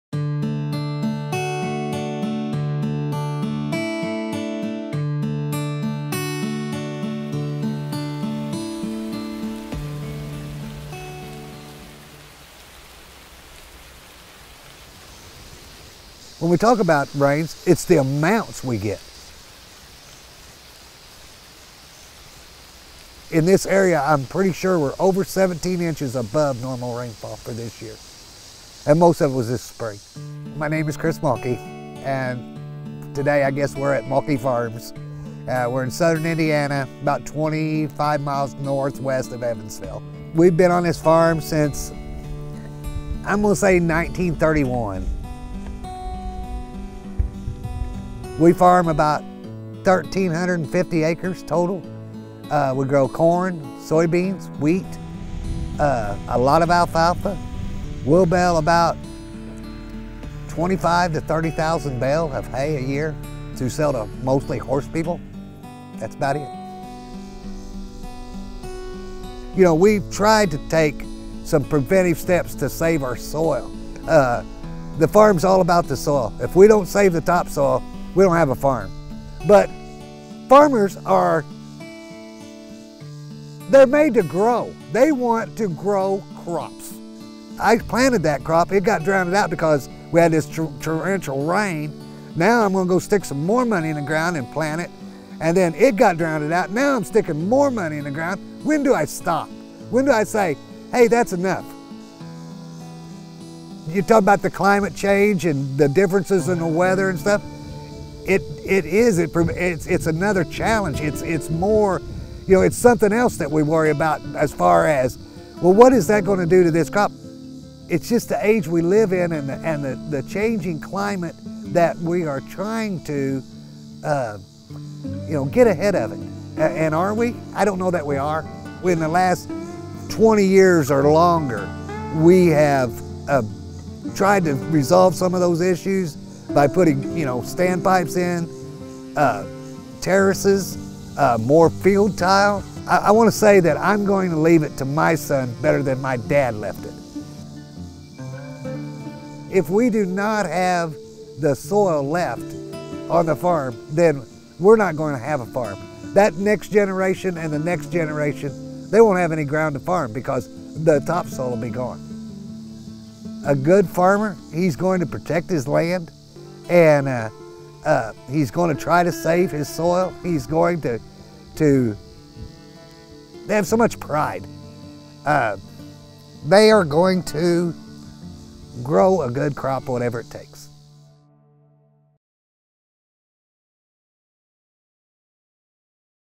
Stories of Change: Climate + Soil
Increased rainfall is challenging soil health, and without healthy soil, our farms are in danger. Chris Mulkey, third-generation producer, shares his perspective on climate change and journey to preserve his family’s land for generations to come.